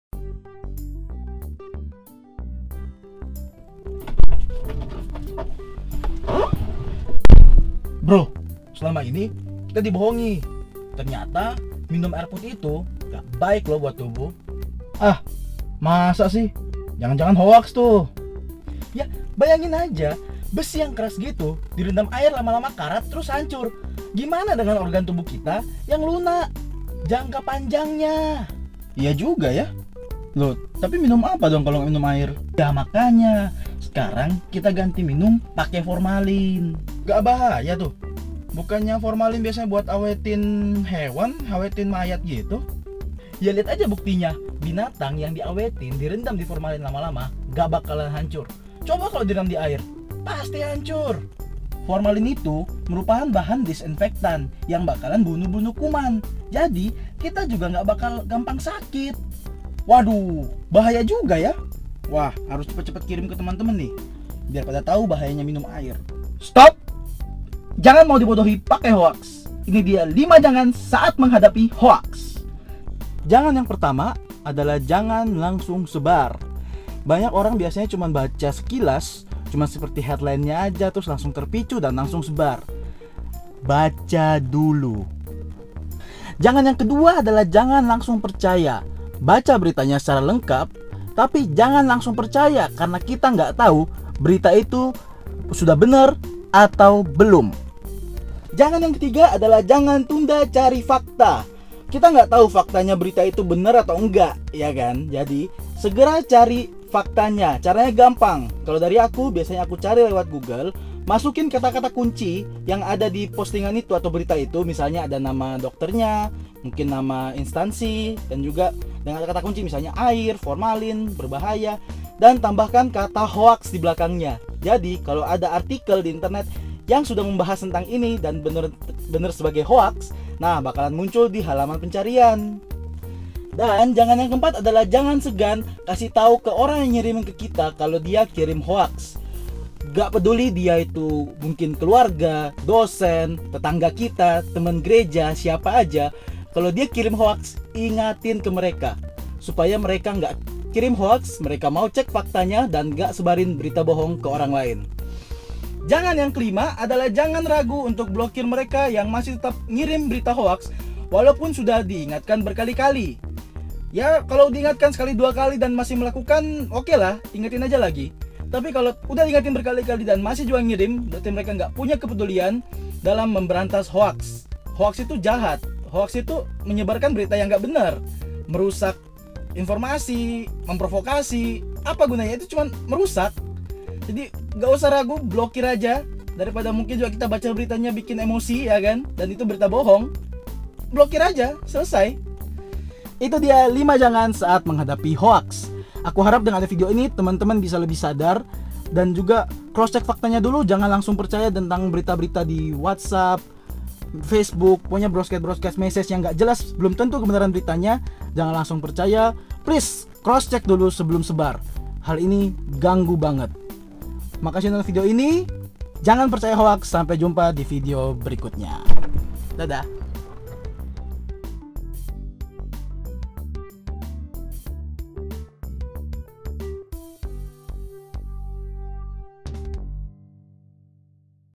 5 JANGAN SAAT MENGHADAPI HOAX
Belakangan ini berita-berita hoax semakin banyak beredar, dan menurutku itu ganggu banget. Makanya aku buat video ini, yang walaupun gak bagus-bagus amat tapi pengen sharing ke teman-teman semua gimana sih menghadapi hoax. Supaya kita bisa jadi pengguna internet yang bijak dan pintar.
Silakan nonton videonya, semoga bermanfaat. Kalau boleh share juga ke teman-teman yang lain supaya semu bisa lebih sadar bagaimana menghadapi hoax. Terima kasih.